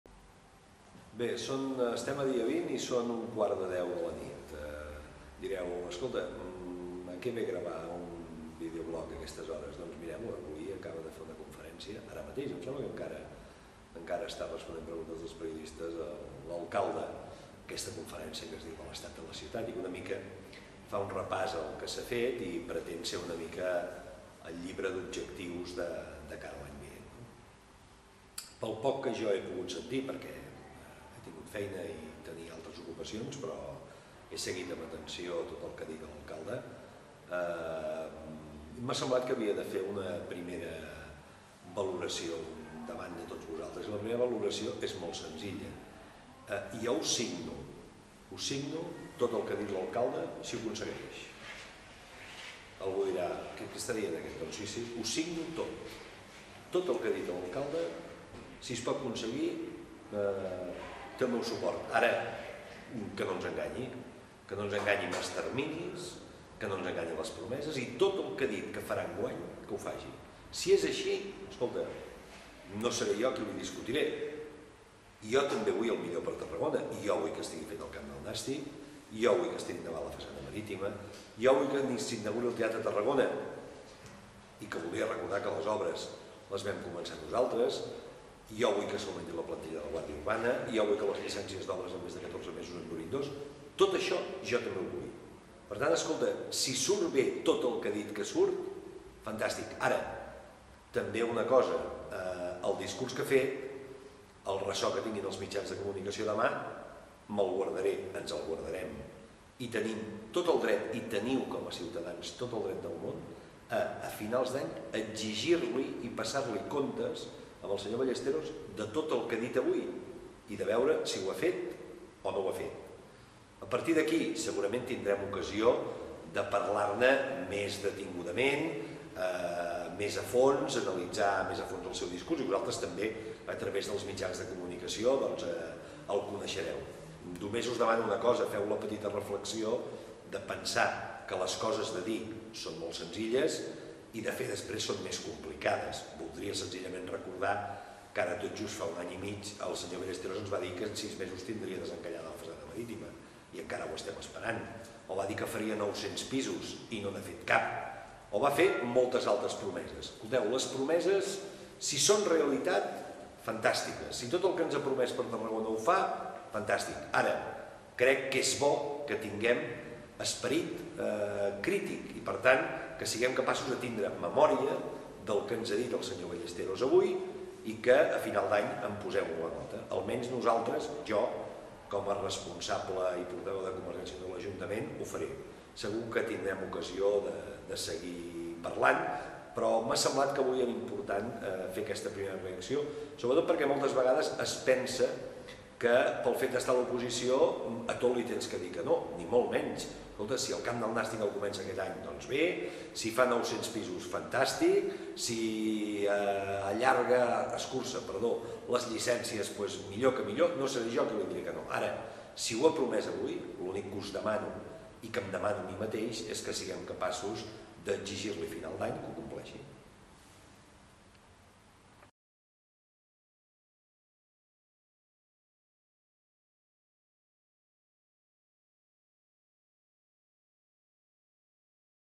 Videobloc 20 de gener de 2009 - L'estat de la ciutat
Aregio ens parla sobre el discurs de l'alcalde sobre l'estat de la ciutat